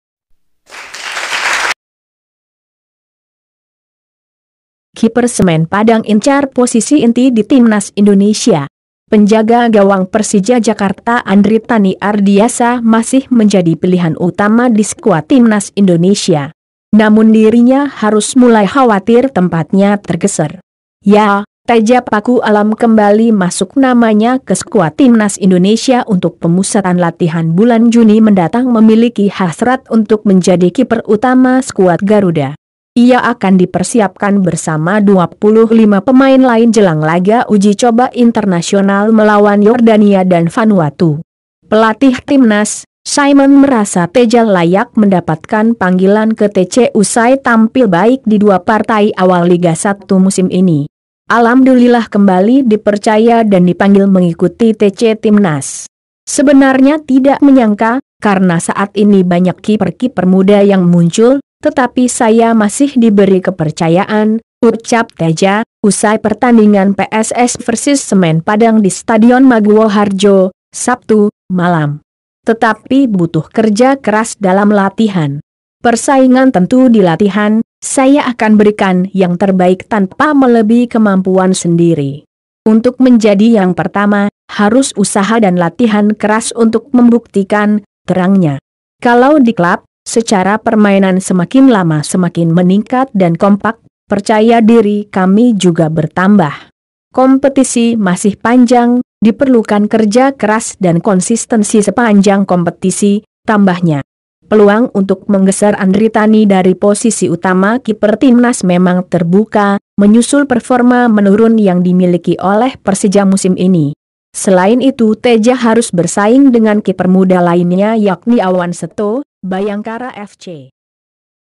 Kiper Semen Padang Incar Posisi Inti di Timnas Indonesia ?
Penjaga gawang Persija Jakarta Andritany Ardhiyasa masih menjadi pilihan utama di skuat Timnas Indonesia. Namun dirinya harus mulai khawatir tempatnya tergeser.

Ya, Teja Paku Alam kembali masuk namanya ke skuat Timnas Indonesia untuk pemusatan latihan bulan Juni mendatang memiliki hasrat untuk menjadi kiper utama skuat Garuda. Ia akan dipersiapkan bersama 25 pemain lain jelang laga uji coba internasional melawan Yordania dan Vanuatu.

Pelatih Timnas, Simon  merasa Teja layak mendapatkan panggilan ke TC usai tampil baik di dua partai awal Liga 1 musim ini.

"Alhamdulillah kembali dipercaya dan dipanggil mengikuti TC timnas. Sebenarnya tidak menyangka, karena saat ini banyak kiper-kiper muda yang muncul, tetapi saya masih diberi kepercayaan," ucap Teja, usai pertandingan PSS versus Semen Padang di Stadion Maguwoharjo, Sabtu (25/5), malam.

"Tetapi butuh kerja keras dalam latihan. Persaingan tentu di latihan, saya akan berikan yang terbaik tanpa melebihi kemampuan sendiri. Untuk menjadi yang pertama, harus usaha dan latihan keras untuk membuktikan," terangnya.

"Kalau di klub, secara permainan semakin lama semakin meningkat dan kompak, percaya diri kami juga bertambah. Kompetisi masih panjang, diperlukan kerja keras dan konsistensi sepanjang kompetisi,” tambahnya.

Peluang untuk menggeser Andritany dari posisi utama kiper Timnas memang terbuka, menyusul performa menurun yang dimiliki oleh Persija musim ini. Selain itu Teja harus bersaing dengan kiper muda lainnya yakni Awan Setho (Bhayangkara FC).